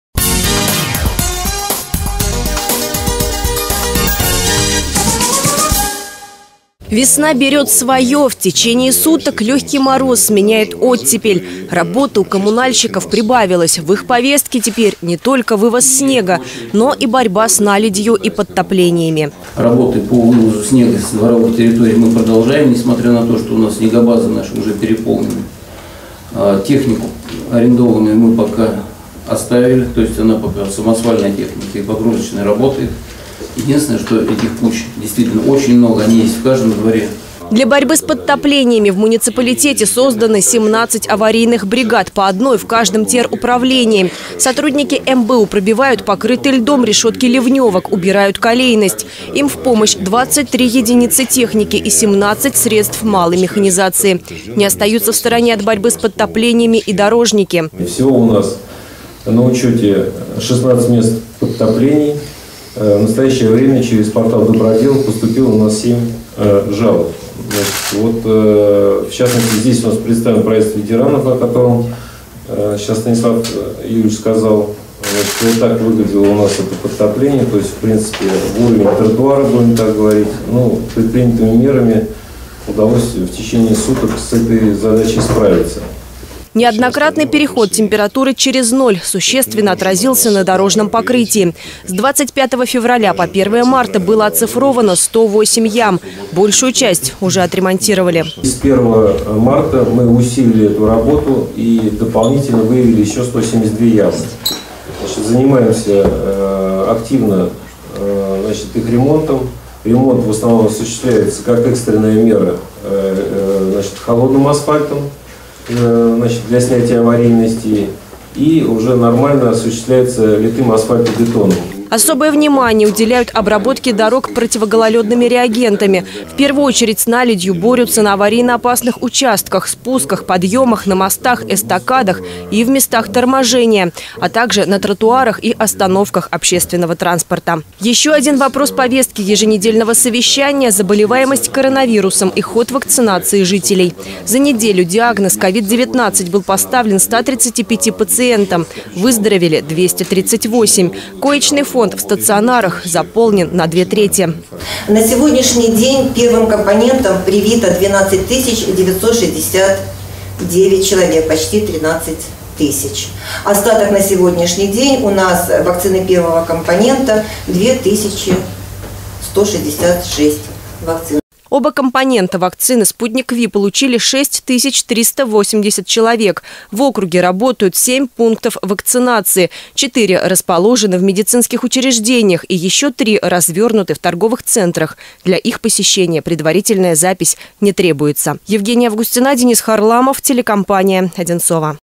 Еженедельное совещание главы
Еженедельное совещание главы муниципалитета в рамках проекта «Выездная администрация» прошло в Звенигороде. В повестке было 9 вопросов.